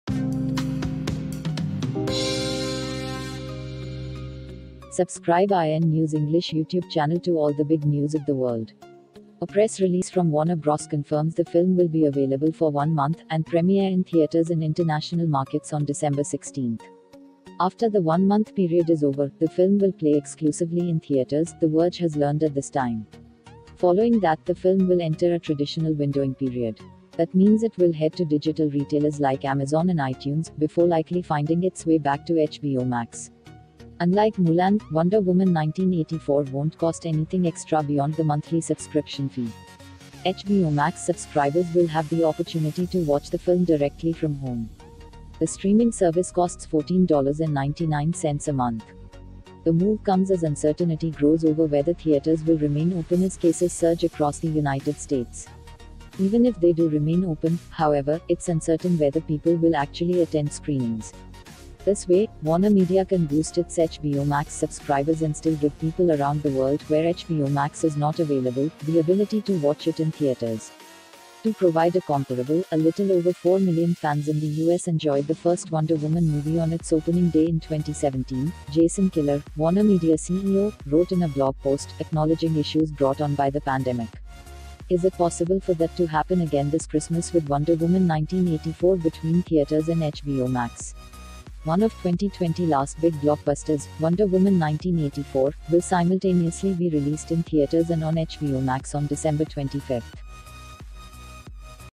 Wonder Woman 1984 will be released on HBO Max the same day it’s in theaters for no extra cost
A press release from Warner Bros. confirms the film will be available for one month, and premiere in theaters in international markets on December 16th. After the one month period is over, the film will play exclusively in theaters, The Verge has learned at this time. Following that, the film will enter a traditional windowing period. That means it will head to digital retailers like Amazon and iTunes, before likely finding its way back to HBO Max.

Unlike Mulan, Wonder Woman 1984 won’t cost anything extra beyond the monthly subscription fee. HBO Max subscribers will have the opportunity to watch the film directly from home. The streaming service costs $14.99 a month. The move comes as uncertainty grows over whether theaters will remain open as cases surge across the United States. Even if they do remain open, however, it’s uncertain whether people will actually attend screenings. This way, WarnerMedia can boost its HBO Max subscribers and still give people around the world (where HBO Max is not available) the ability to watch it in theaters.

“To provide a comparable, a little over four million fans in the U.S. enjoyed the first Wonder Woman movie on its opening day in 2017,” Jason Kilar, WarnerMedia CEO, wrote in a blog post, acknowledging issues brought on by the pandemic. “Is it possible for that to happen again this Christmas with Wonder Woman 1984 between theaters and HBO Max?”

One of 2020’s last big blockbusters, Wonder Woman 1984, will simultaneously be released in theaters and on HBO Max on December 25th.